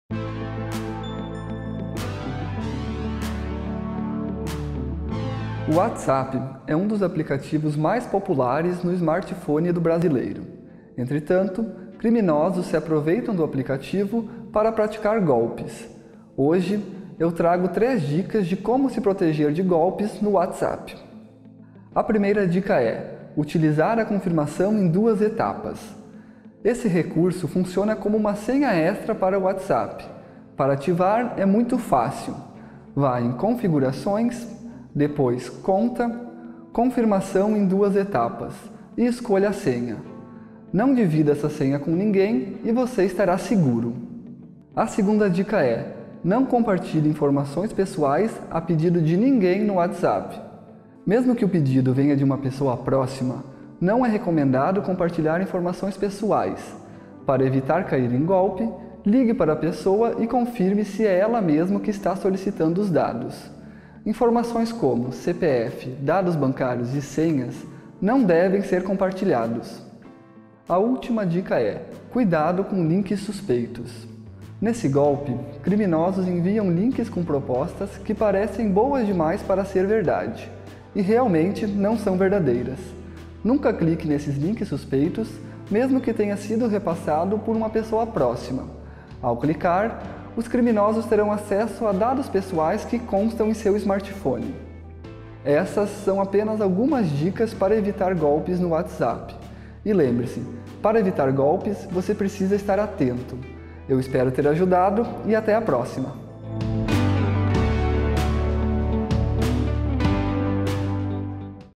Cuidados na internet - Como se prevenir de golpes pelo WhatsApp
📳 Muitos golpes virtuais acontecem por meio do aplicativo de mensagens WhatsApp.

✅ Hoje mostramos algumas medidas que podem ajudar a evitar esse tipo de armadilha.